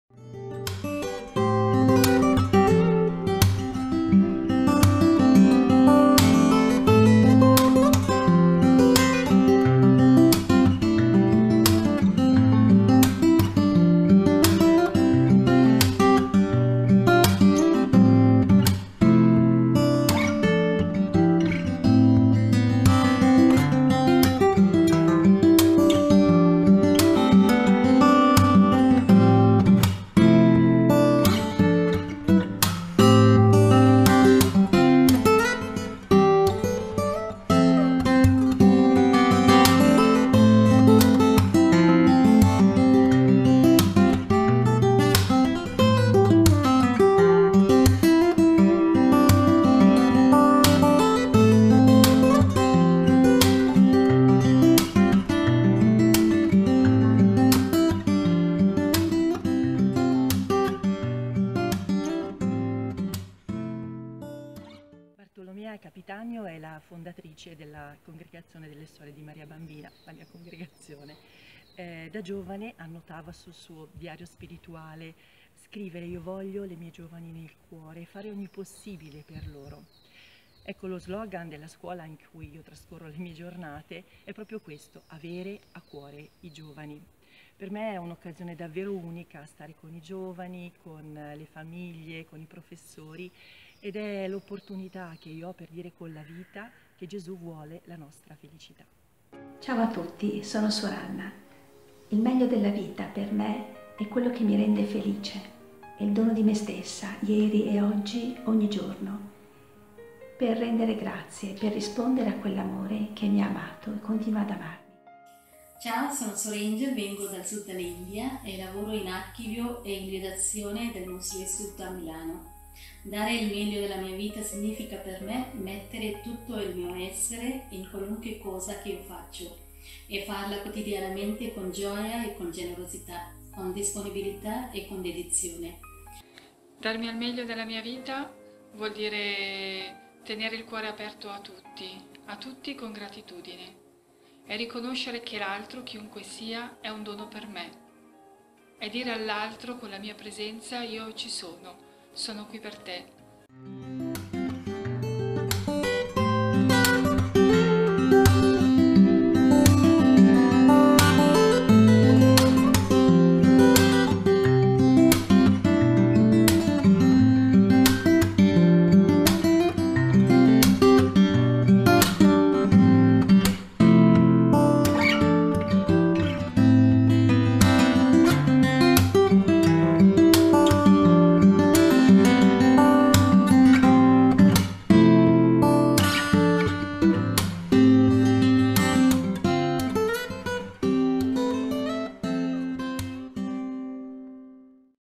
"Datevi al Meglio della Vita!" - la Vocazione si racconta: Le Suore di Maria Bambina
Dopo la serie di video delle scorse settimane, in occasione della Giornata Mondiale di Preghiera per le Vocazioni 2020, ogni martedì continueremo a pubblicare un breve vide-racconto "inedito", una Vocazione diversa che si presenta con la sua storia, i suoi consigli e la sua voglia di "darsi al Meglio!".